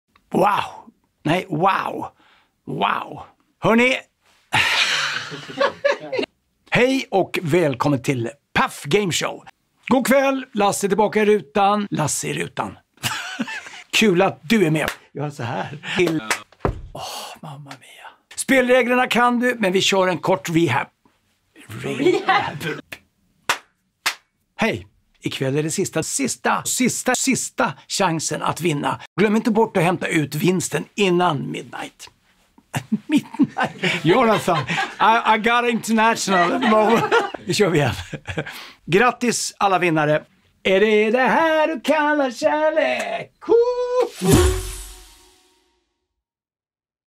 Lasse Holm Paf Game Show – Bloopers!
Roliga bloopers med Lasse Holm från vår quizapp - ladda ner appen gratis och quizza loss. Du kan vinna VIP-biljetter till schlagerfinalen!